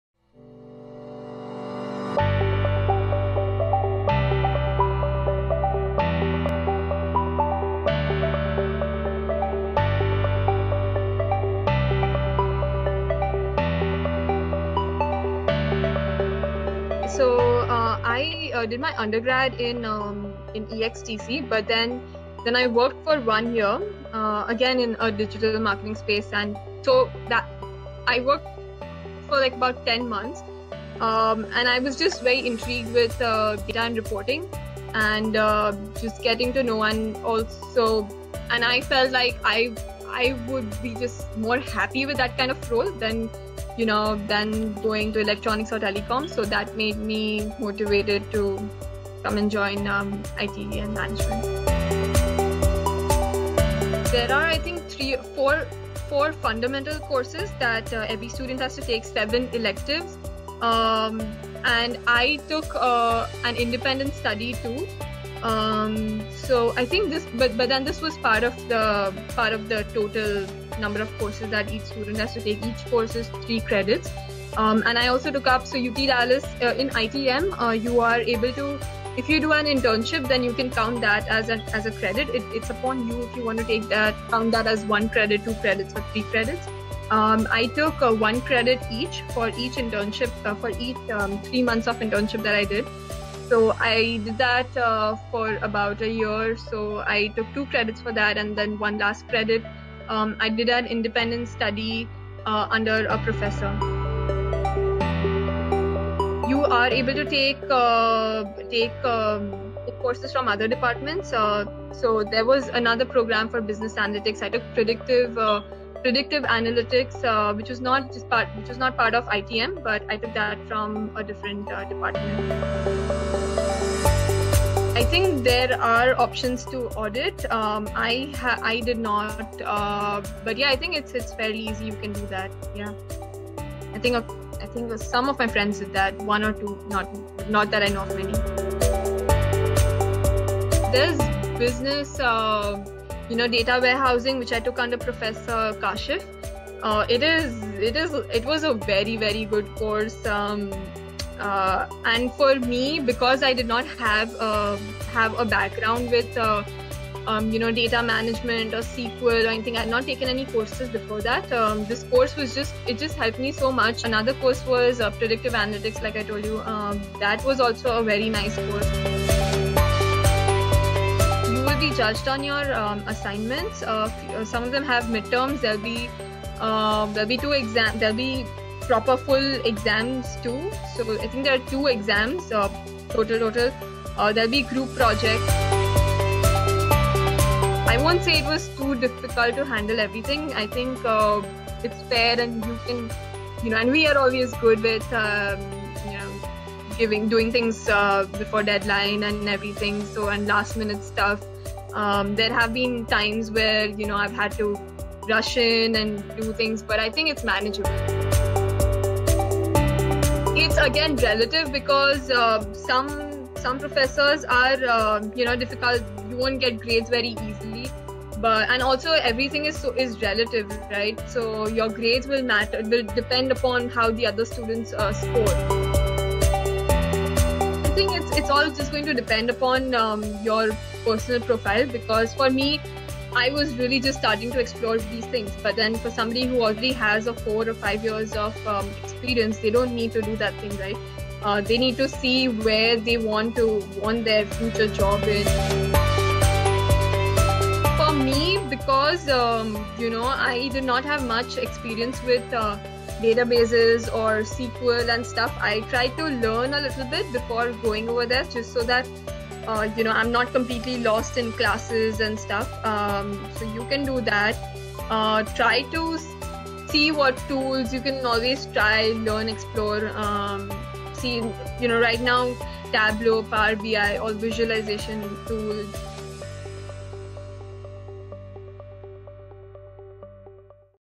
Information Technology & Management at the University of Texas at Dallas | Academics at UT Dallas
Meet Richa, a Collegepond Alumnus and graduate of the University of Texas at Dallas. She  shares with you crucial firsthand information about academics at UTD... right from insight into the course, class diversity and profile, credit requirement, coping with academic pressure, and more